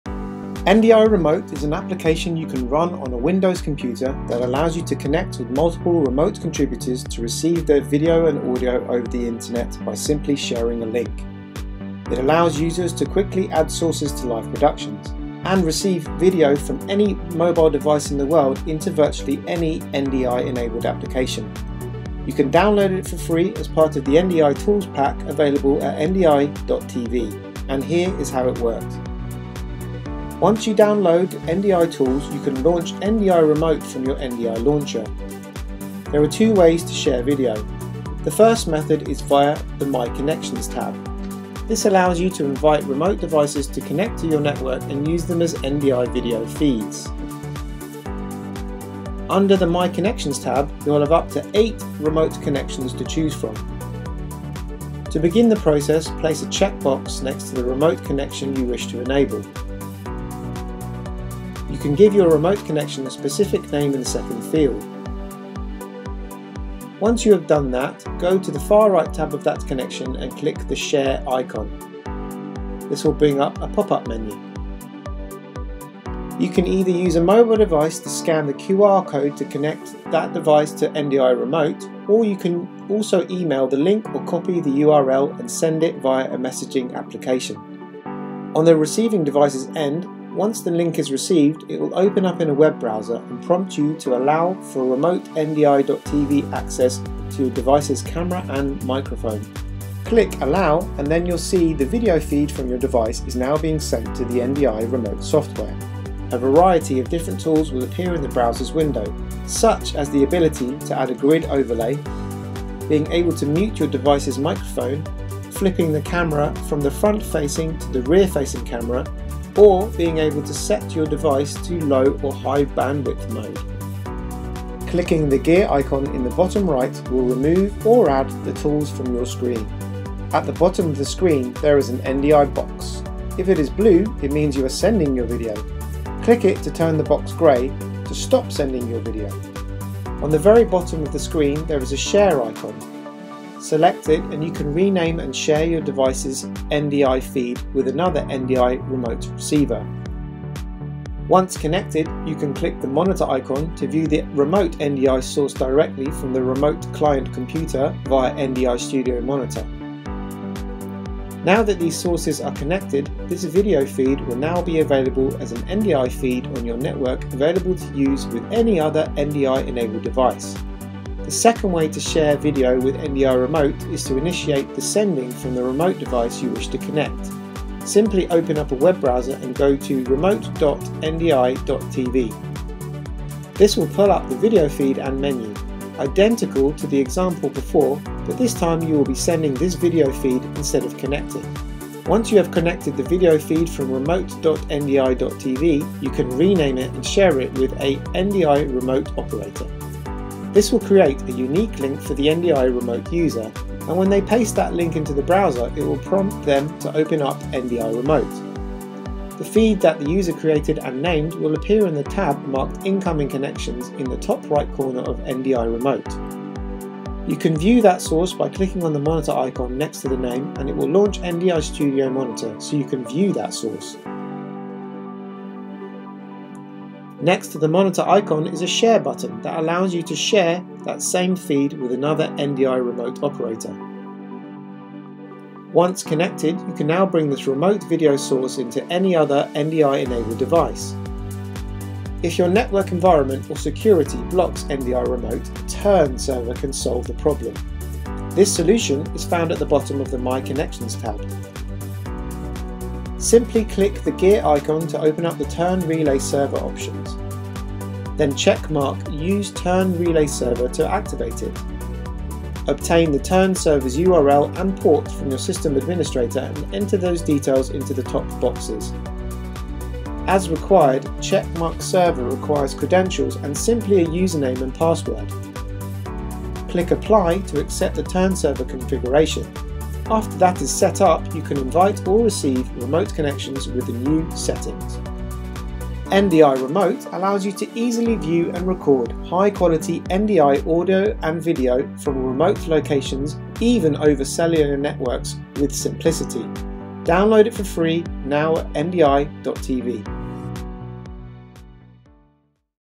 NDI Remote Explained
Securely connect with multiple remote contributors to receive their video and audio over the internet by simply sharing a link.